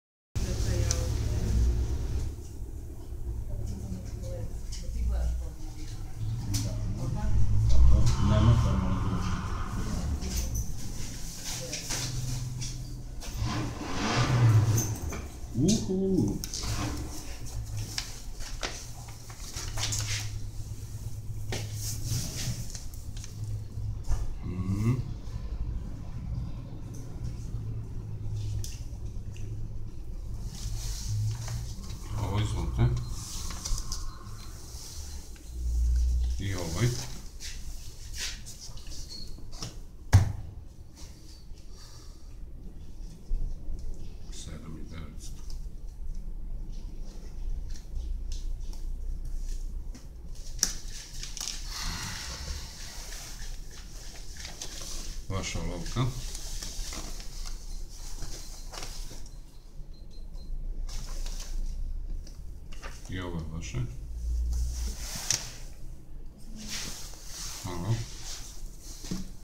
Potpisivanje dve platne liste
Zaposleni u "Ćurdiću" potpisuju dve platne liste, jednu blanko, a drugu sa sumom